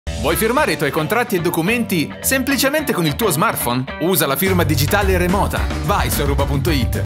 Firma Digitale Remota con OTP Mobile 7sec - Aruba.it
Firma i tuoi contratti e documenti semplicemente con il tuo smartphone. Acquista la firma digitale remota di Aruba.it e potrai firmare con valore legale, in modo semplice e veloce. Scopri di più su